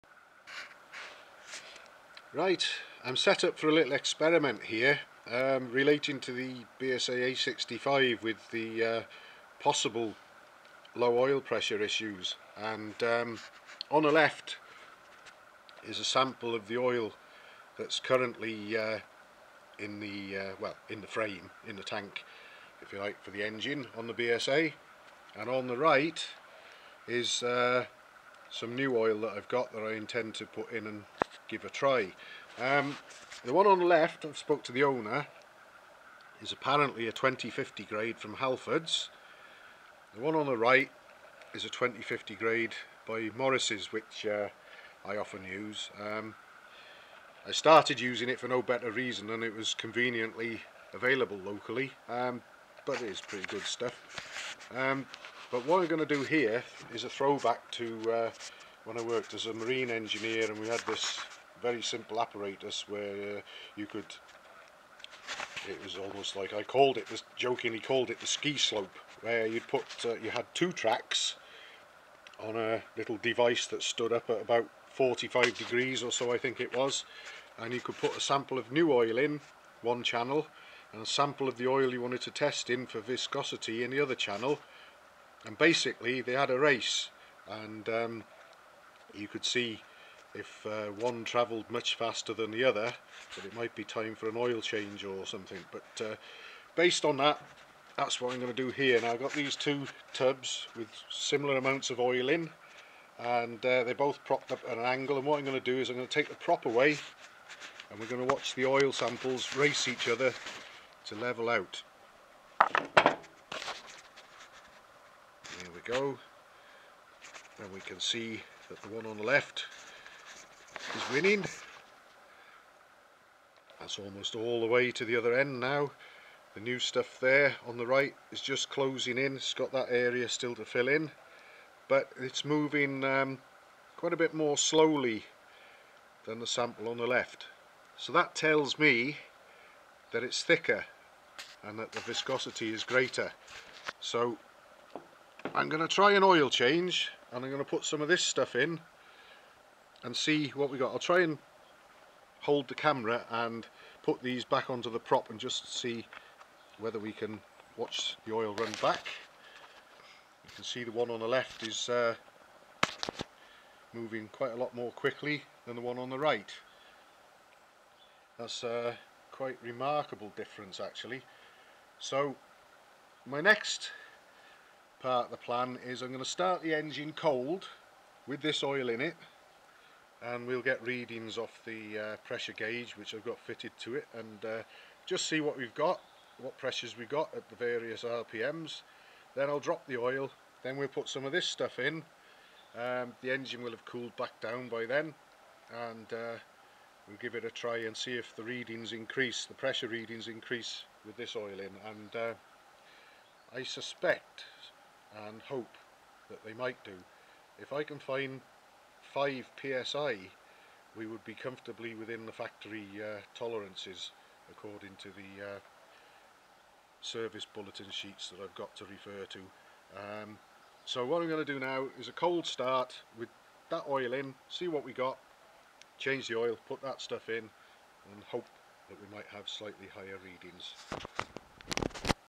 Oil viscosity comparisons of two brands of 20/50 grade [one very slightly used]
I made this comparison, not so much to compare one brand directly against another, as this may not have been entirely fair seeing as one had done a very low mileage. I made this comparison while investigating a slightly lower than ideal oil pressure from a recently rebuilt engine and the oil in the engine appeared to be a little thinner than I would have expected, and compared with the brand I frequently use, it was indeed thinner. There was a notable increase in the engine's oil pressure, but that story is ongoing, with lots of video footage coming together for when the whole story can hopefully be told.